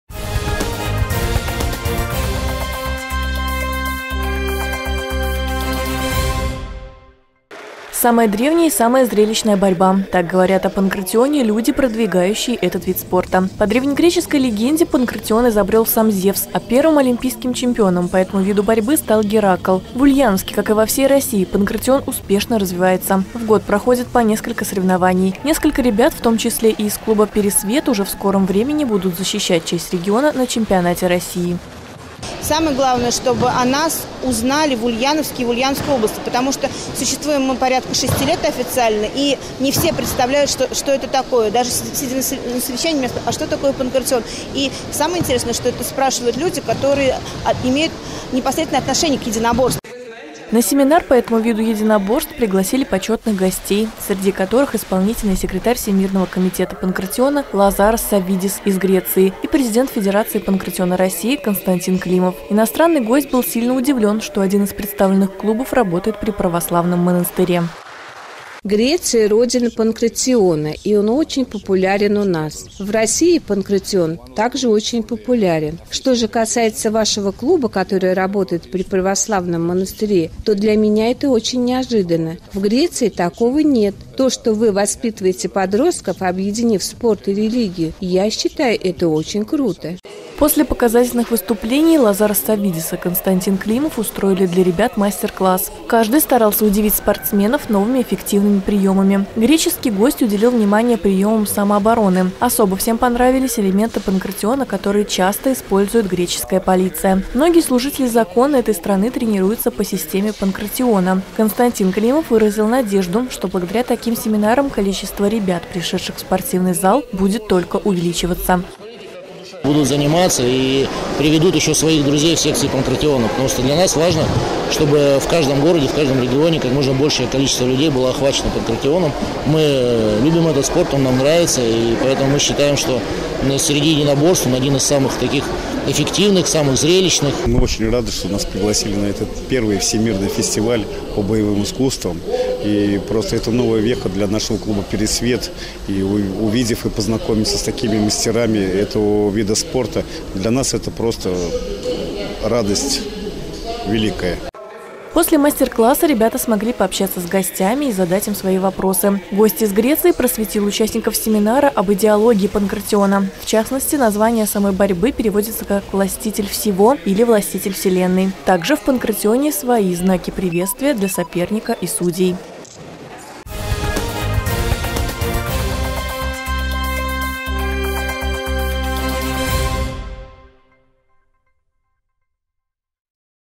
Самая древняя борьба. Ульяновским ребятам показали мастер-класс по панкратиону
В ФОК Тхэквандо, в рамках Всемирного фестиваля боевых искусств ТАФИСА, прошёл семинар по спортивной борьбе панкратион.